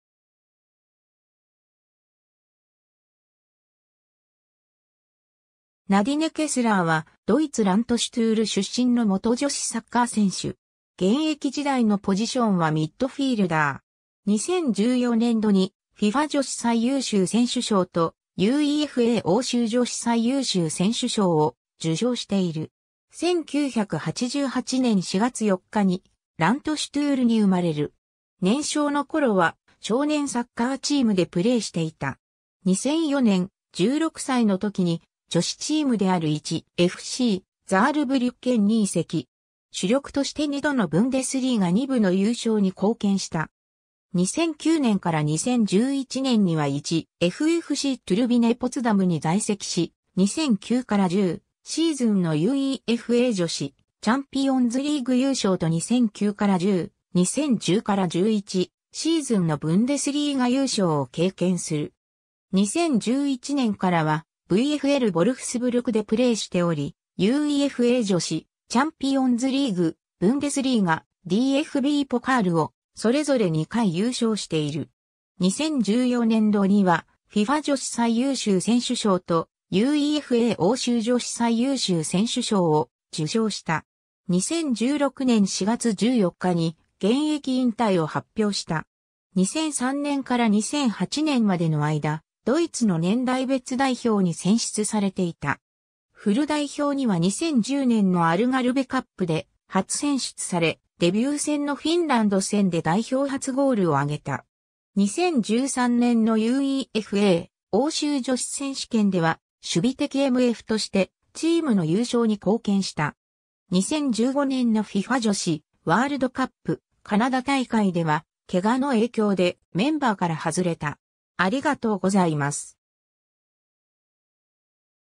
ナディネ・ケスラー
#1.FFCトゥルビネ・ポツダムの選手
ナディネ・ケスラー（Nadine Keßler、1988年4月4日 - ）は、ドイツ・ラントシュトゥール出身の元女子サッカー選手。
2014年度にFIFA女子最優秀選手賞とUEFA欧州女子最優秀選手賞を受賞している。
 1988年4月4日にラントシュトゥールに生まれる。
年少の頃は少年サッカーチームでプレーしていた。
 2004年、16歳の時に女子チームである1.FCザールブリュッケンに移籍。
主力として2度のブンデスリーガ・2部の優勝に貢献した。
 2009年から2011年には1.FFCトゥルビネ・ポツダムに在籍し、2009-10シーズンのUEFA女子チャンピオンズリーグ優勝と2009-10、2010-11シーズンのブンデスリーガ優勝を経験する。
 2011年からはVfLヴォルフスブルクでプレーしており、UEFA女子チャンピオンズリーグ、ブンデスリーガ、DFBポカールをそれぞれ2回優勝している。
 2014年度にはFIFA女子最優秀選手賞とUEFA欧州女子最優秀選手賞を受賞した。
 2016年4月14日に現役引退を発表した。
 2003年から2008年までの間、ドイツの年代別代表に選出されていた。
 フル代表には2010年のアルガルヴェ・カップで初選出され、デビュー戦のフィンランド戦で代表初ゴールをあげた。
 2013年のUEFA欧州女子選手権では守備的MFとしてチームの優勝に貢献した。
 2015年のFIFA女子ワールドカップ・カナダ大会では、怪我の影響でメンバーから外れた。